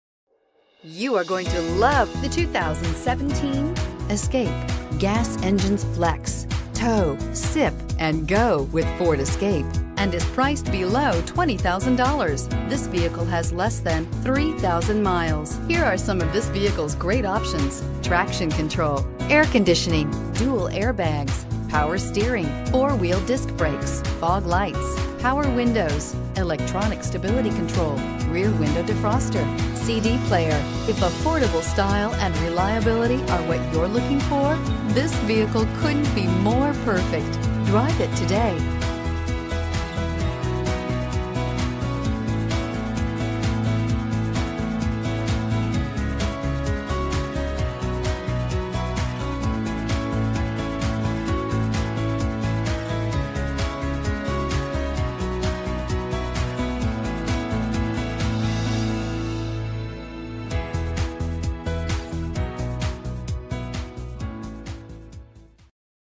2017 Ford Escape HUE84178
2017 FORD ESCAPE Morrow, GA
Stock# HUE84178

Odometer is 13979 miles below market average! 30/23 Highway/City MPG

Reviews:

* Optional 2.0-liter turbocharged engine provides quick acceleration; high-tech features include Sync 3 infotainment, Apple CarPlay and Android Auto; responsive steering and carlike handling around turns. Source: Edmunds